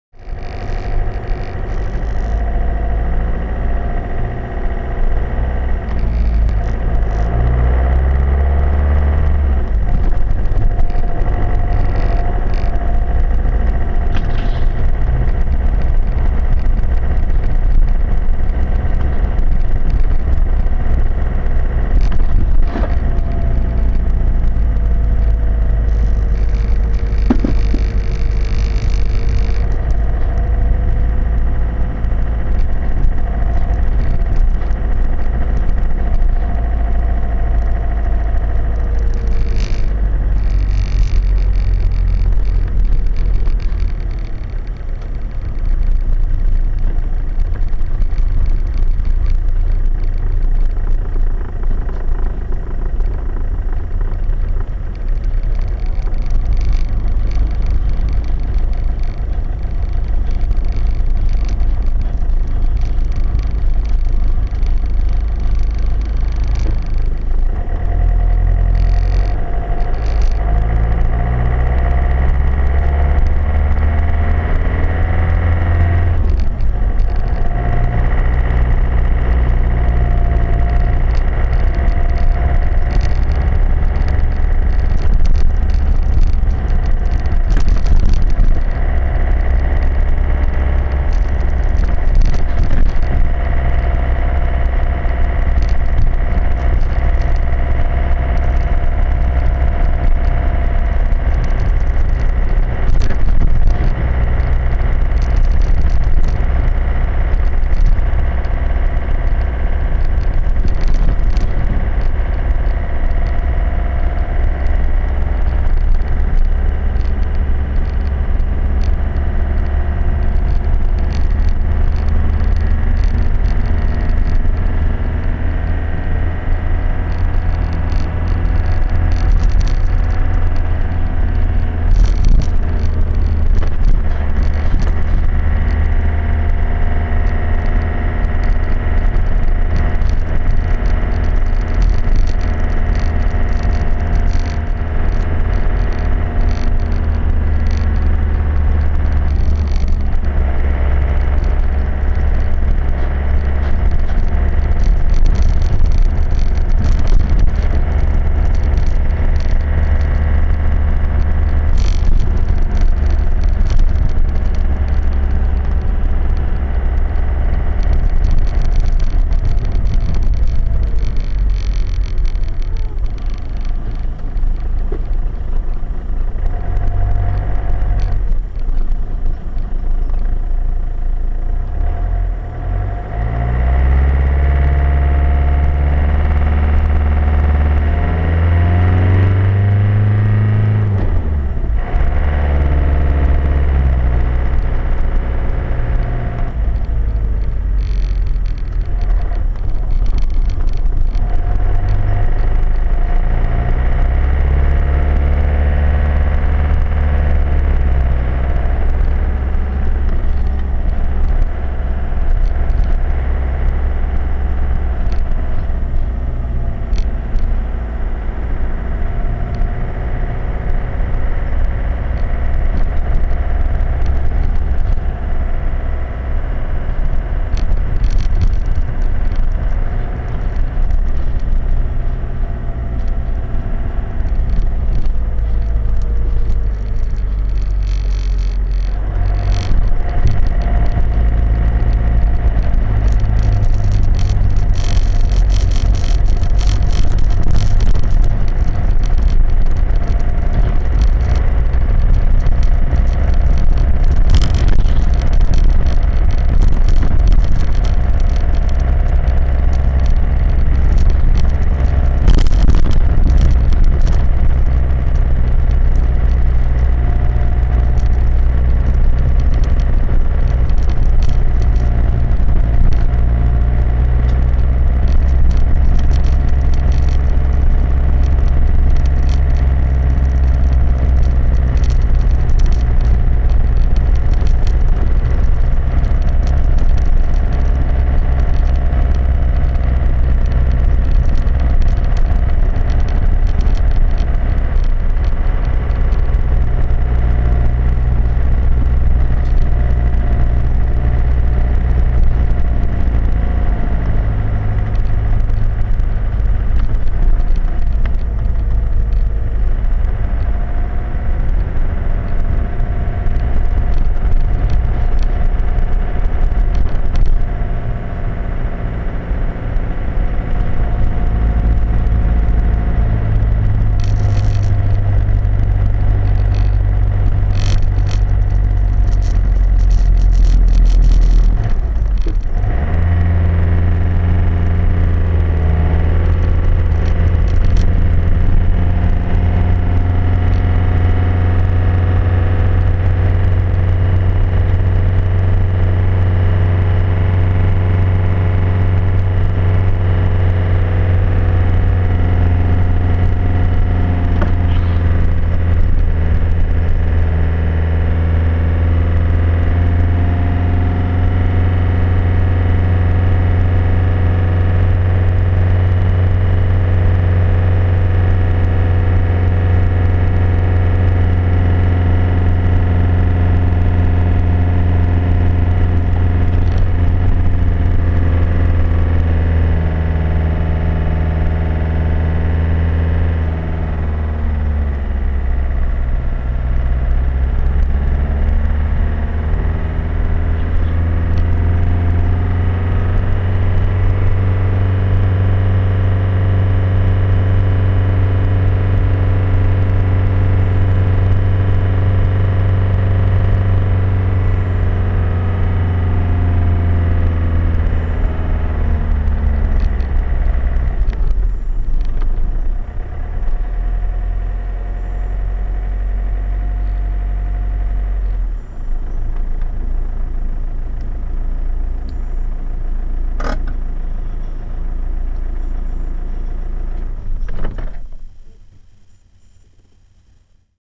På jakt etter en plass i solen
Kjørte med boblien min, en Hymer 564, for å finne en plass i solen å parkere. Jeg bor i Harstad og siden jeg ville kjøre motoren varm før jeg parkerte, ble det en liten omvei.
Solen har ikke et sort hull i sentrum slik det kan se ut i filmen. Det sorte skyldes nok noe  videoteknisk.